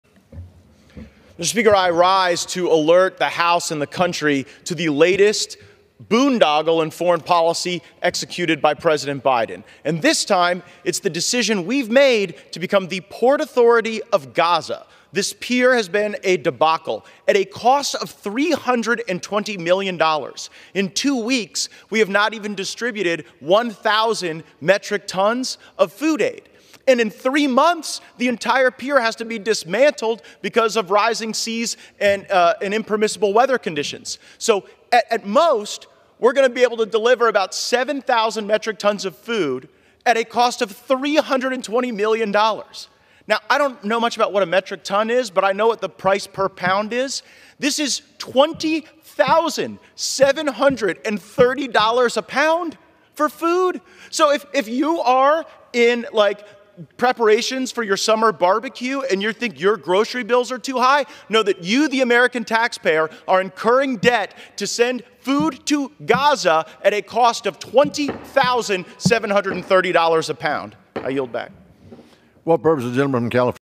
BREAKING NEWS: Matt Gaetz Absolutely Excoriates Biden Over Shocking Cost Of Gaza Pier
In remarks on the House floor, Rep. Matt Gaetz (R-FL) tore into President Biden's spending on the Gaza aid pier.